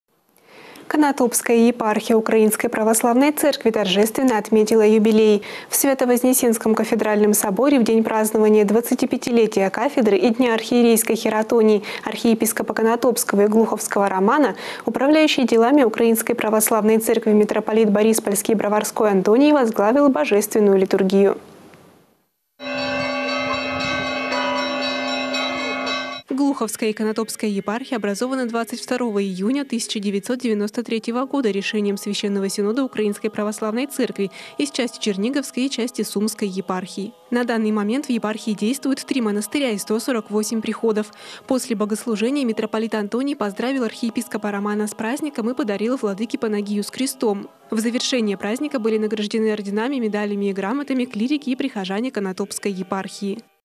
Конотопская епархия торжественно отметила юбилей своего архипастыря
Конотопская епархия Украинской православной церкви торжественно отметила юбилей. В Свято-Вознесенском кафедральном соборе в день празднования 25-летия кафедры и дня архиерейской хиротонии архиепископа Конотопского и Глуховского Романа, управляющий делами Украинской Православной Церкви митрополит Бориспольский и Броварской Антоний возглавил Божественную литургию.